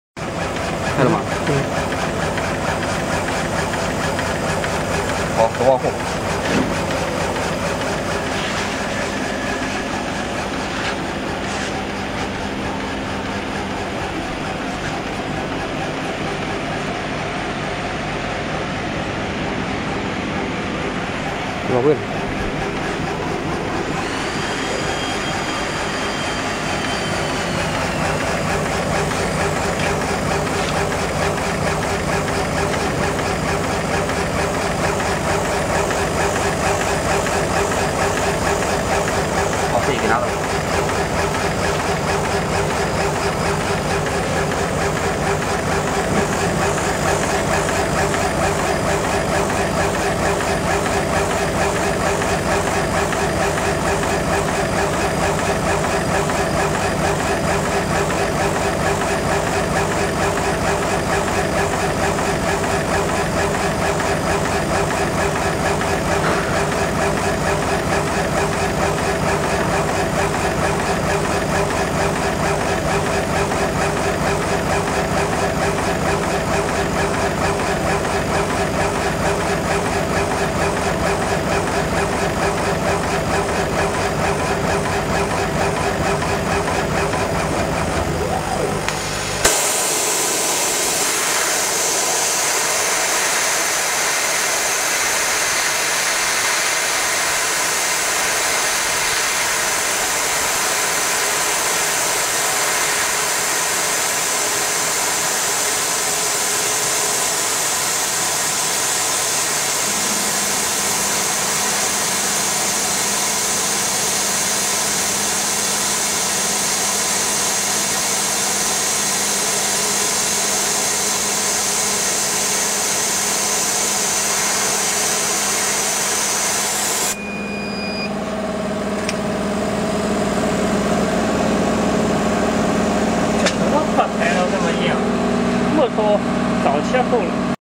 Jinan ruofen laser engraving machine on pvc
laser machine 80W cut wood from Jinan ruofen laser machinery.